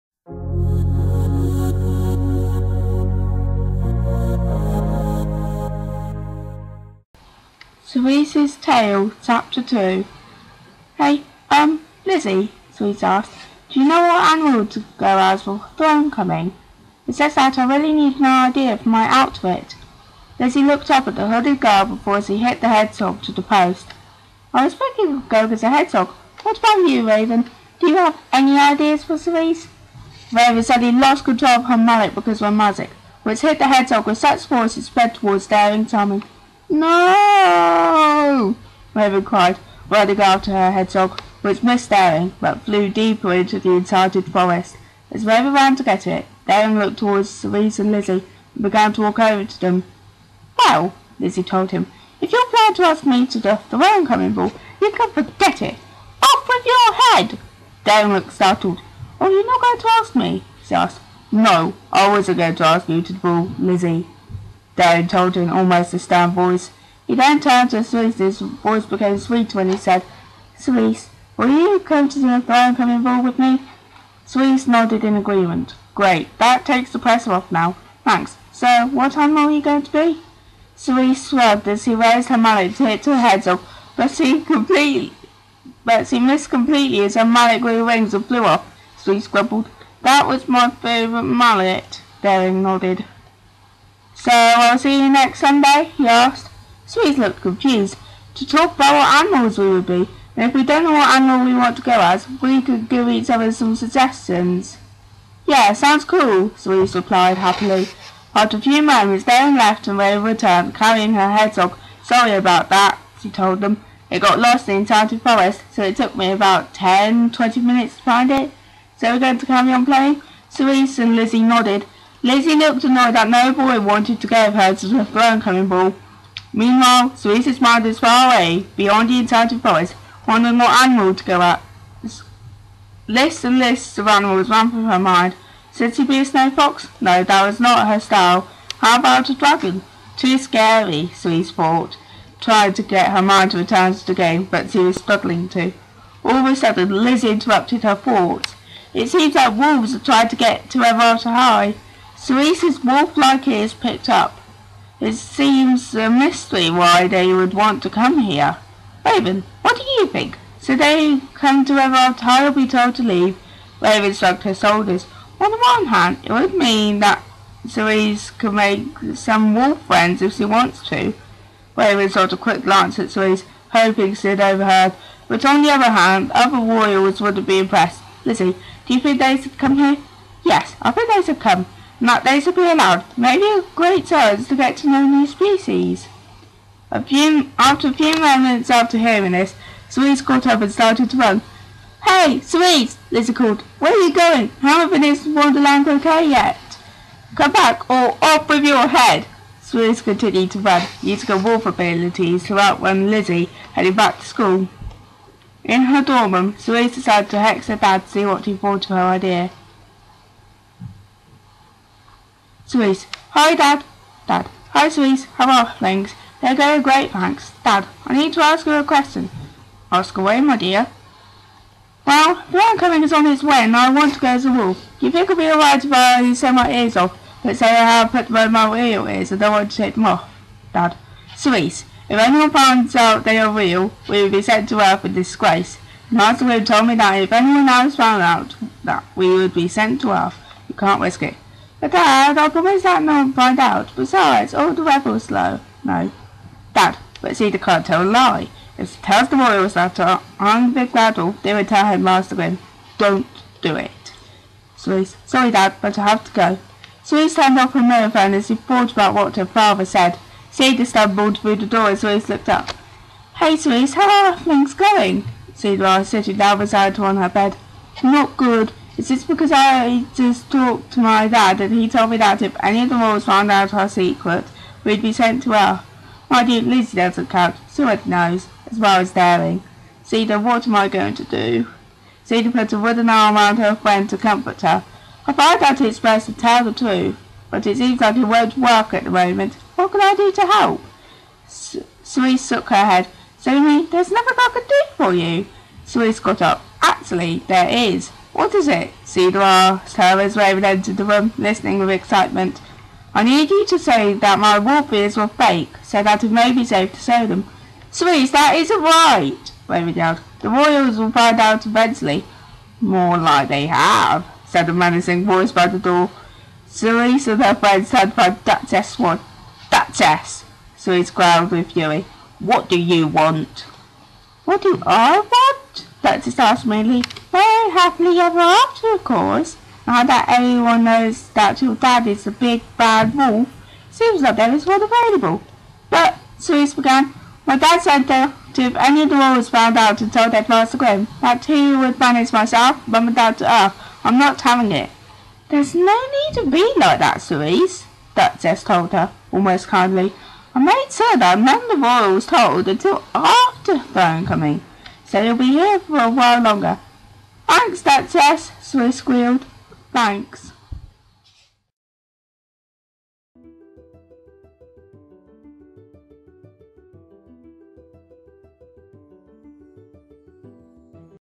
Cerise's tale audio chapter 2
At Ever After High, Thronecoming is just around the corner. Daring asked Cerise to go with him and Cerise agrees. However, there is a big problem round the corner...